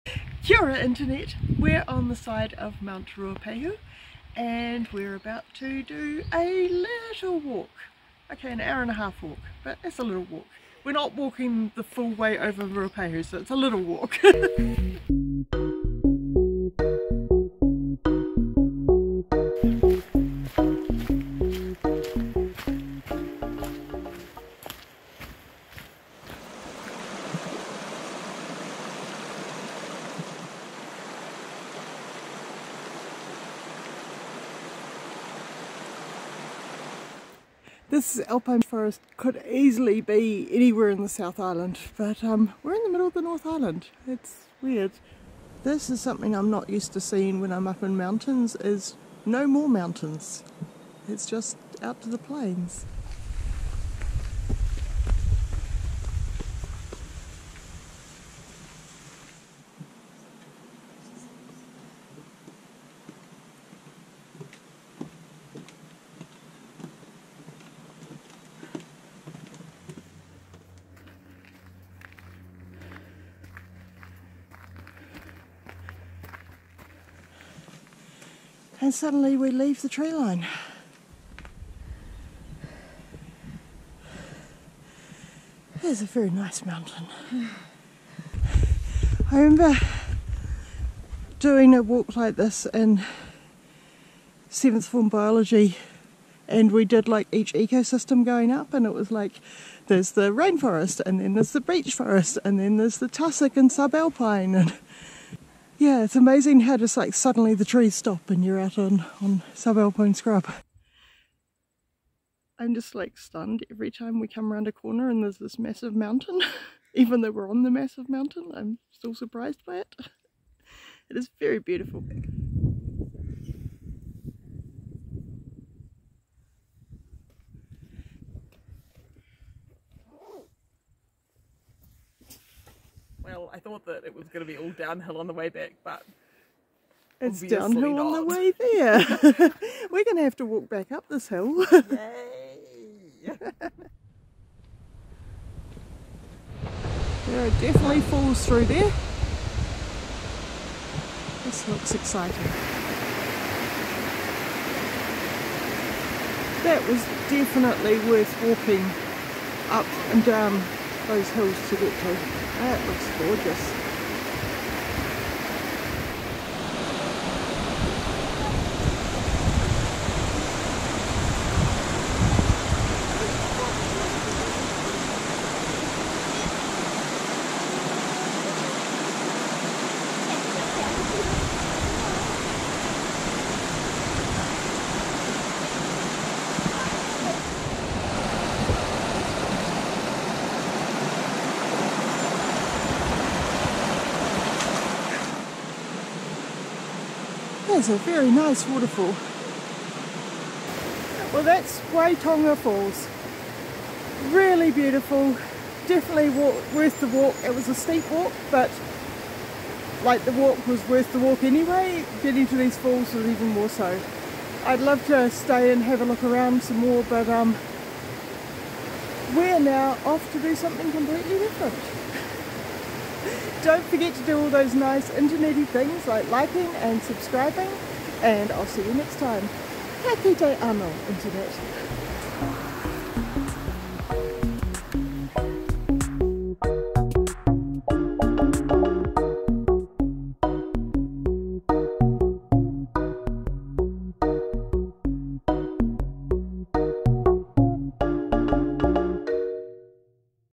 Travel Local: Waitonga Falls Walk, Mt Ruapehu
During our North Island road trip last year, we did one of the many walks on Mt Ruapehu, to Waitonga Falls.